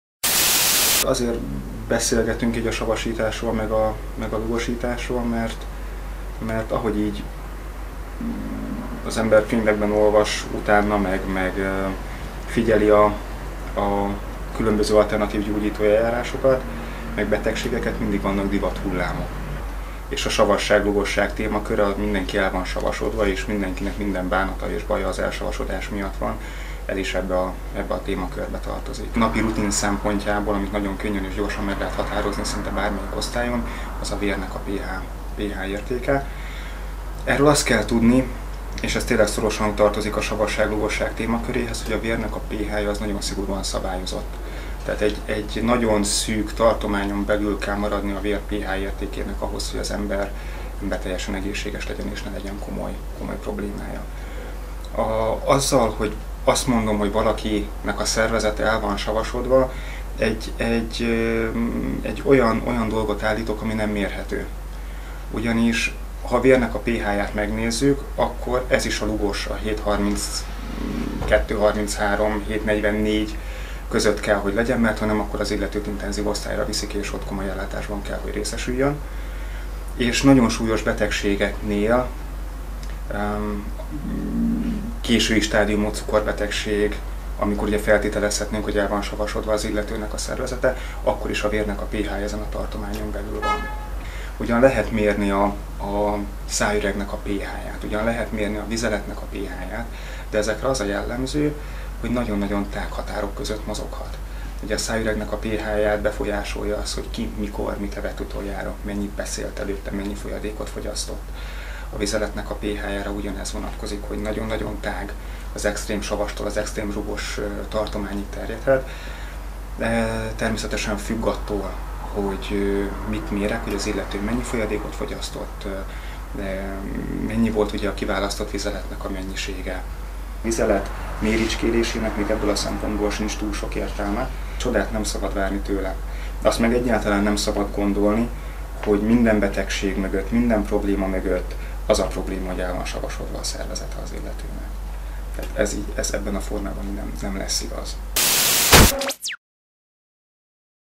[Nem Titok!] Tényleg minden problémám oka az hogy el vagyok savasodva?
Dr. Antal Péter Miklós beszél a mostanában rettentően divatos "elsavasodásról". Elmondja hogy miért nem helyesek ezek a mérések és miért nem igaz hogy minden problémánaknak ez az okozója.